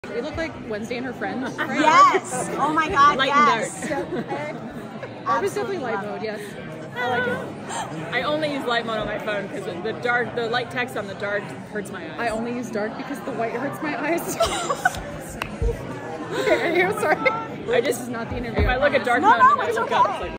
Teaser- Lindsay Jones and Barbara Dunkelman: WonderCon 2023
Just a little teaser of two of my favorite people I got to interview at WonderCon!  Tune in April 25th to watch Justice League X RWBY!